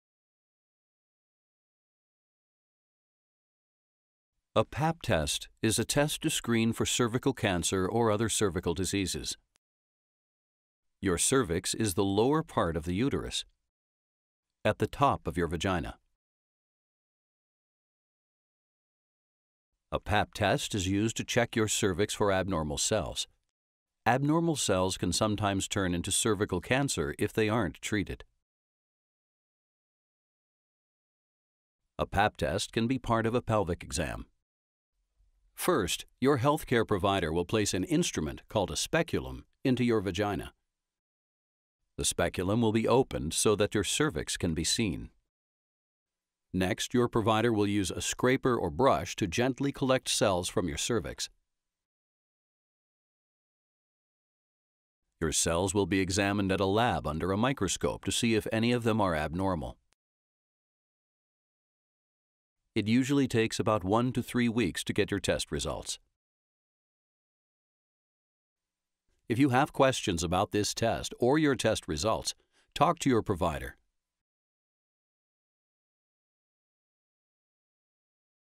Pap Test
A Pap test is a test to screen for cervical cancer or other cervical diseases. This video explains the reasons why it may be done and describes how the test is performed.

ANH17192